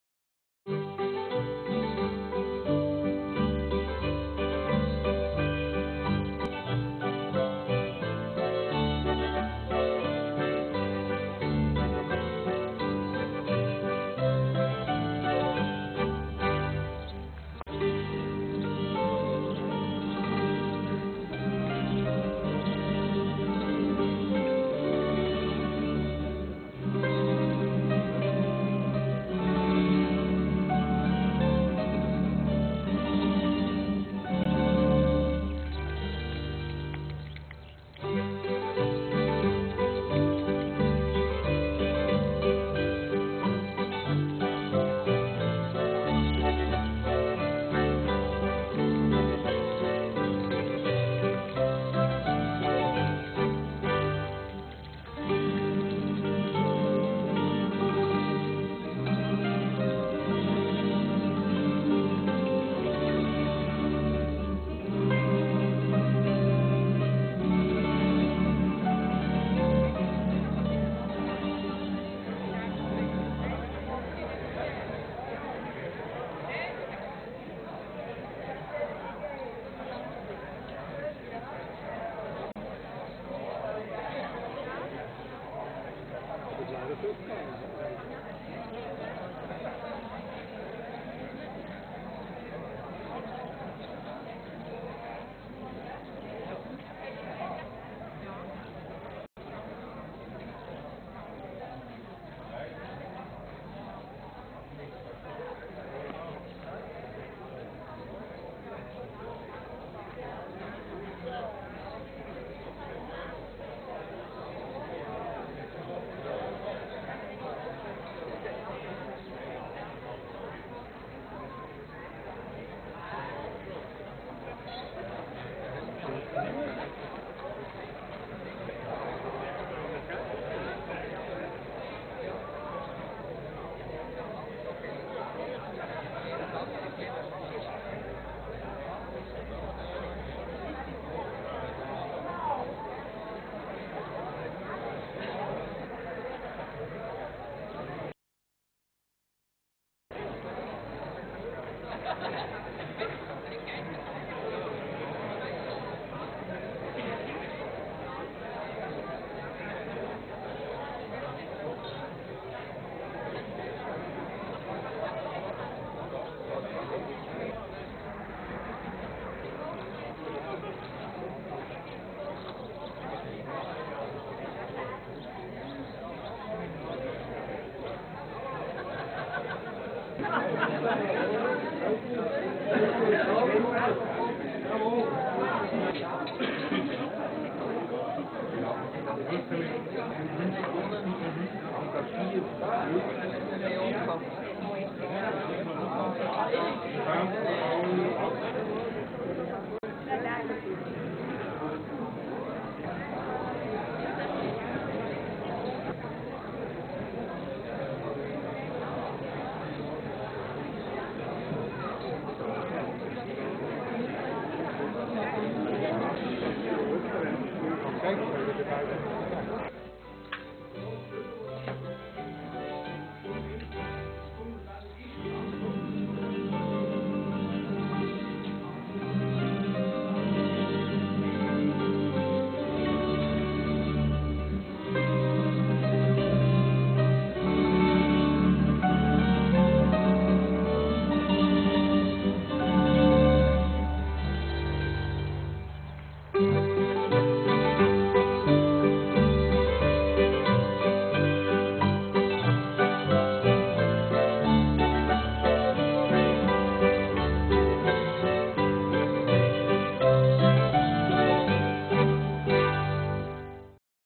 Apéro im Schloss Toffen
Das Sommertheater Gürbetal hat seine Première im Schloss Toffen gefeiert. Ein Film von Wolfgang Brunner.